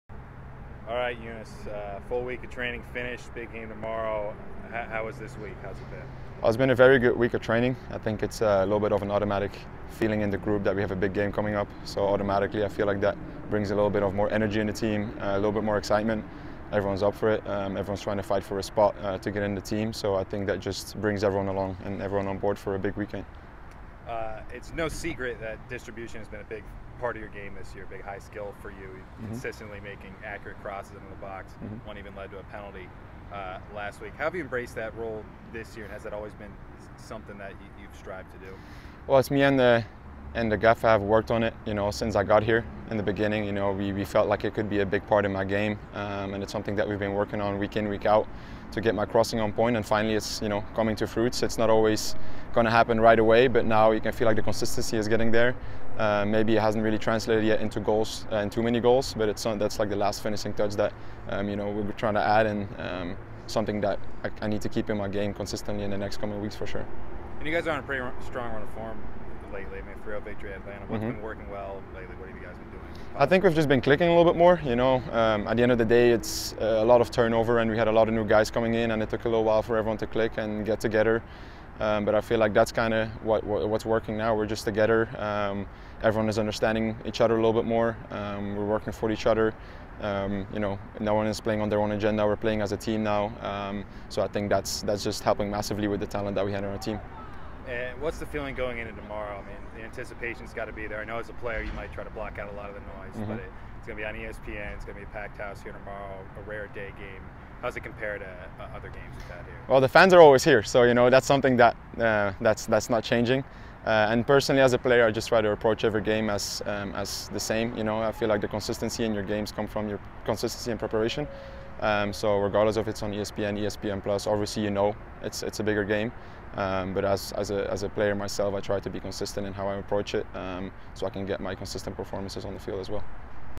Boudadi: "We're all on board for a big weekend."
Younes Boudadi gives his thoughts from the training pitch ahead of Hartford's home match against Louisville City FC on Saturday at 1:00 PM.